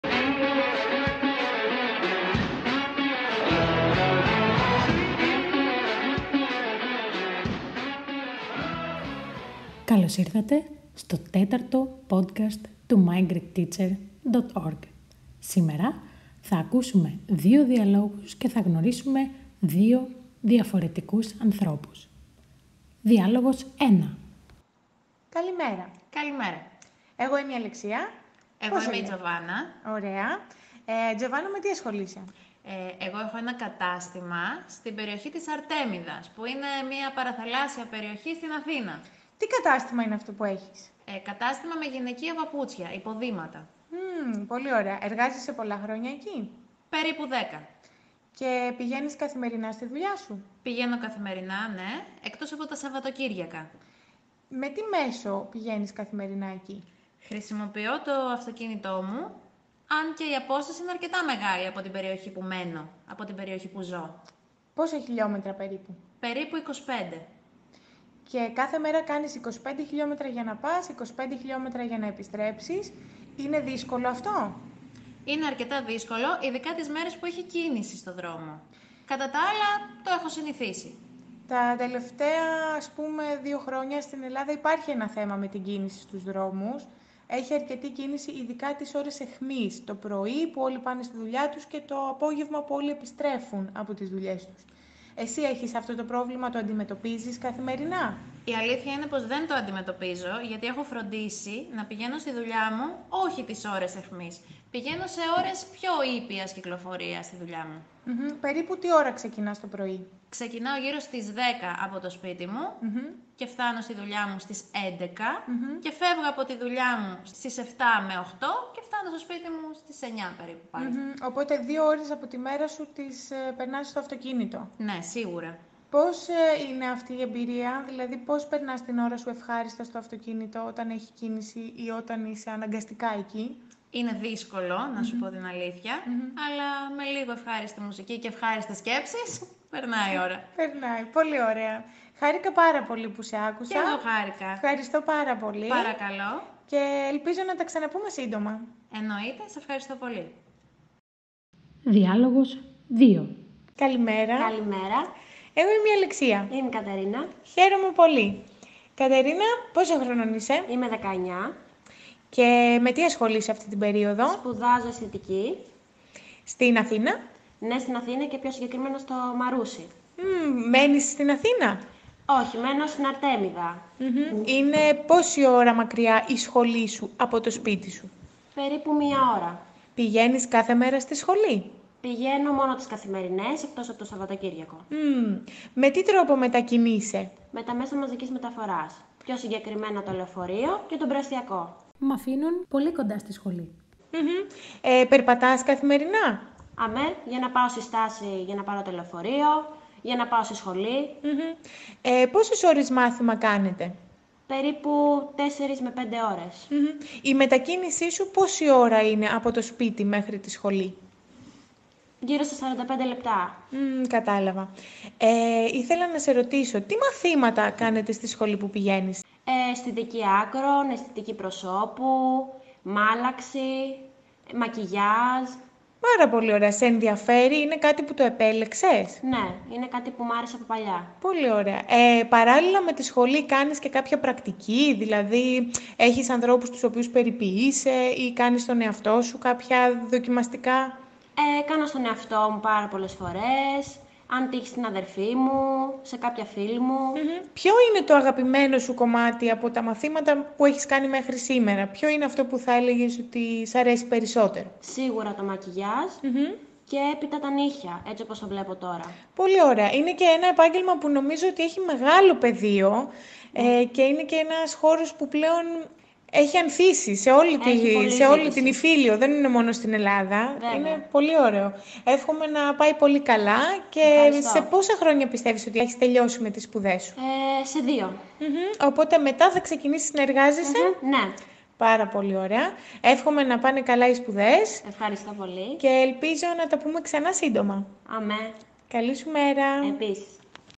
My Greek Teacher- Επεισόδιο 4🧍🏻‍♀️🧍🏽‍♀️Discussion 🇬🇷(A2)
Σε αυτό το επεισόδιο ακούμε δύο διαλόγους και γνωρίζουμε 2 διαφορετικούς ανθρώπους.Καλή διασκέδαση με το τέταρτο επεισόδιο του mygreekteacher.org

- on the first pages the greek text which follows the two dialogues.
- In the following pages you will find the greek text with translation.
- Bonus! On this Podcast there are also exercises for you. Make sure you answer them and send them for feedback.